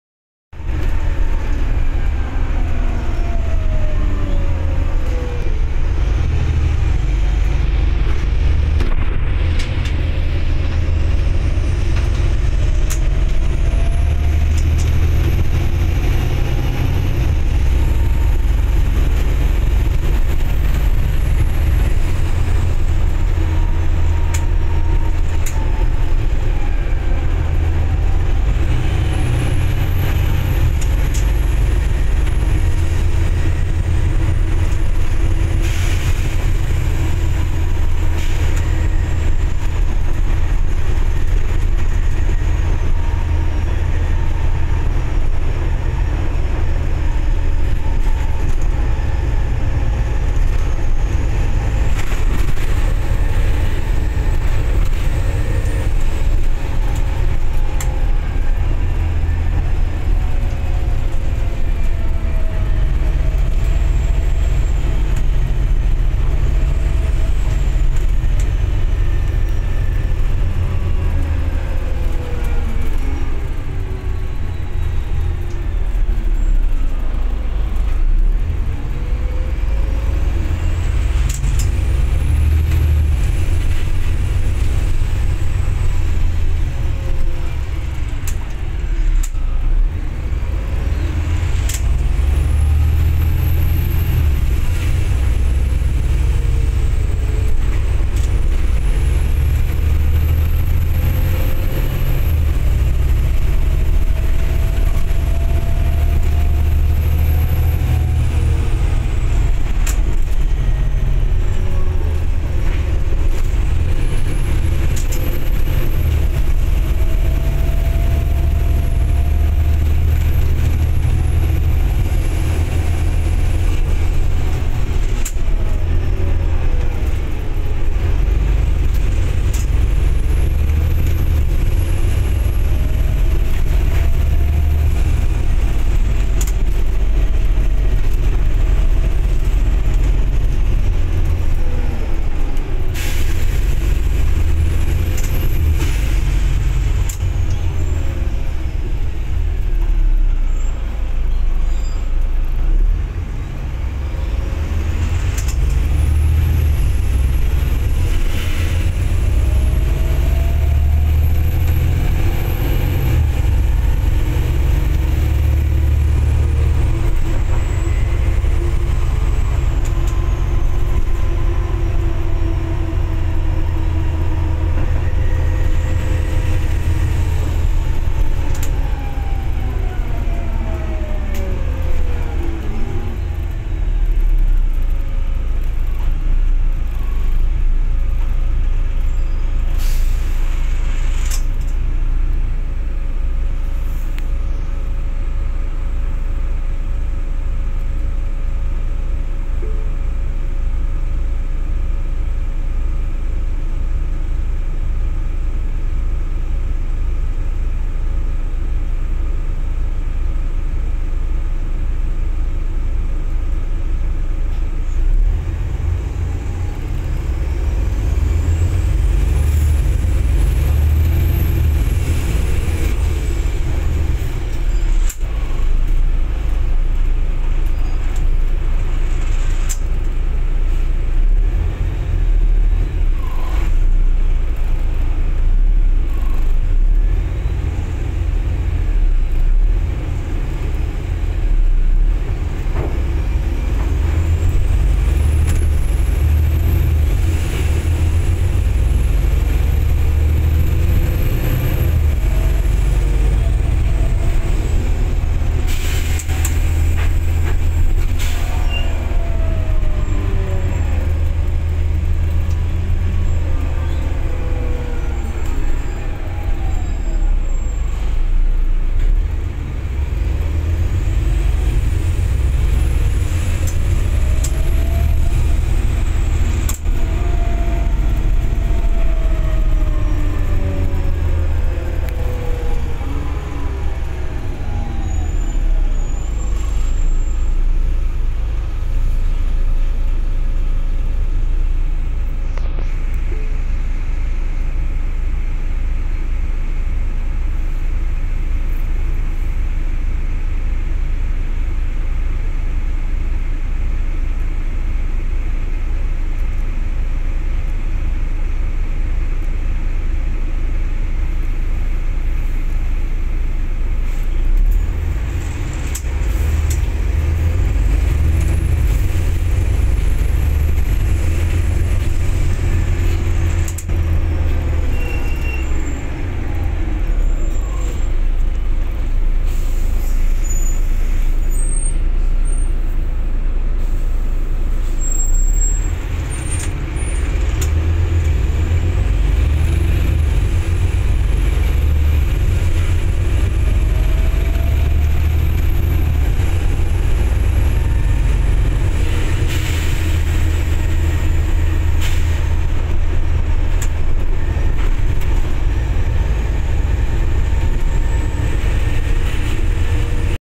Alx 400 (2995)
Seen driving through Comber this afternoon 31/1/20